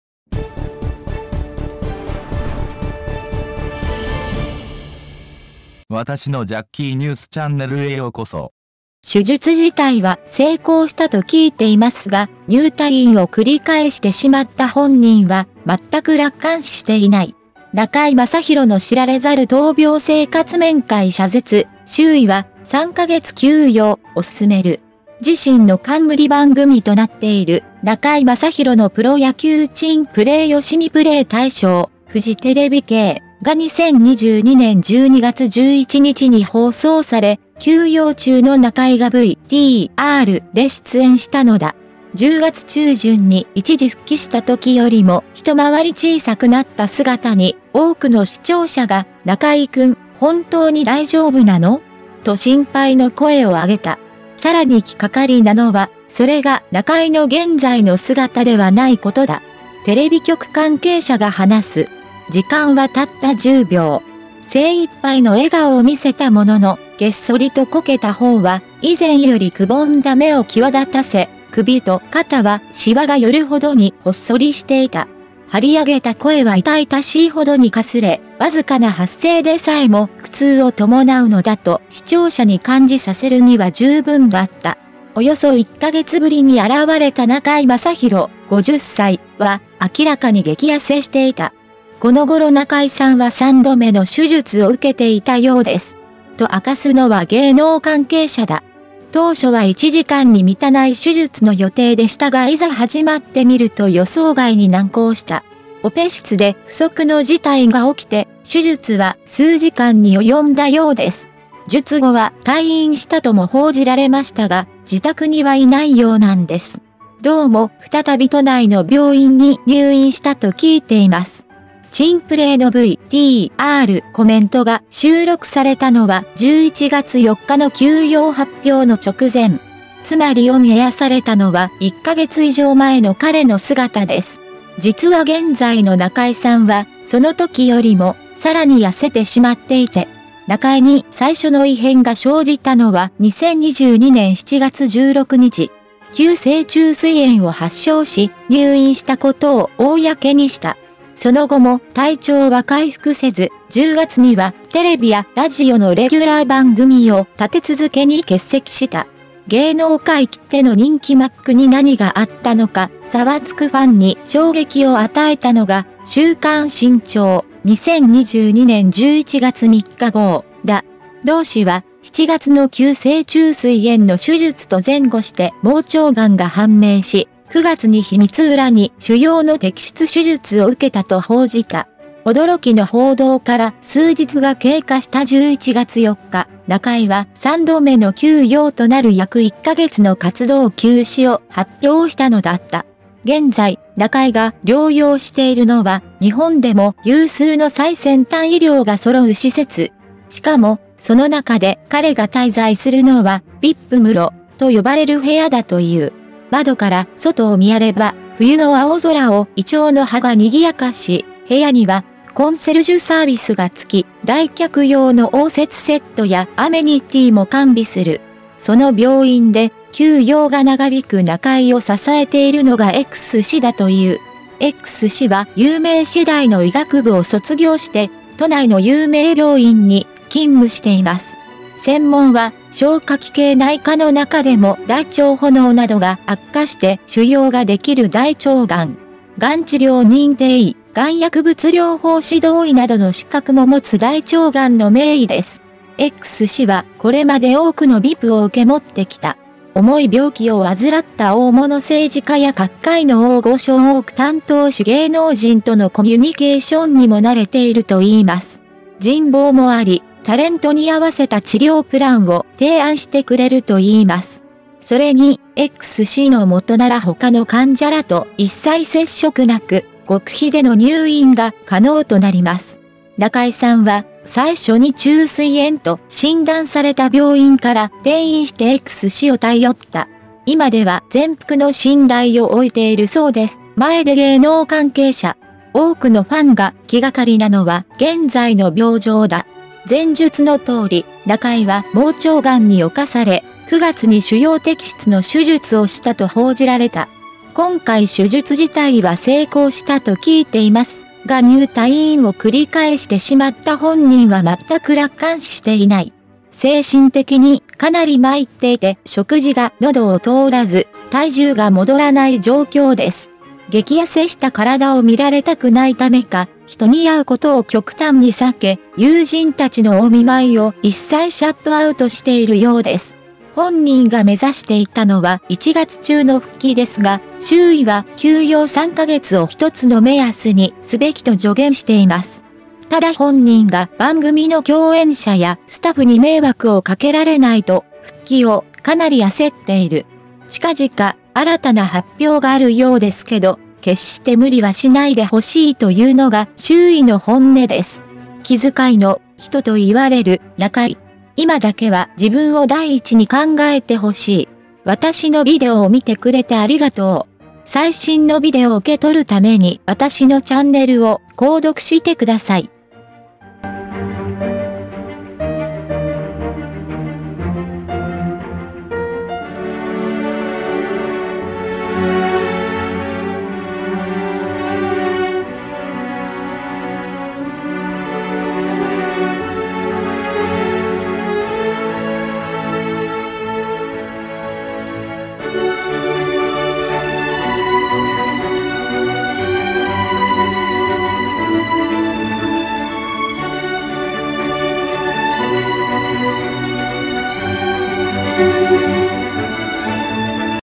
手術自体は成功したと聞いていますが、入退院を繰り返してしまった本人は、まったく楽観視していない。中居正広の知られざる闘病生活 面会謝絶、周囲は「3カ月休養」を勧める。
《動画の内容》
He was aiming to return in January, but people around him have advised that "three months of rest" should be used as a guideline. However, he is in a hurry to return if he does not cause trouble to the co-stars and staff of the program.
SMAP 中居正広 草彅剛 香取慎吾 木村拓哉 稲垣吾郎 J-POPアーティスト#ジャッキーニュース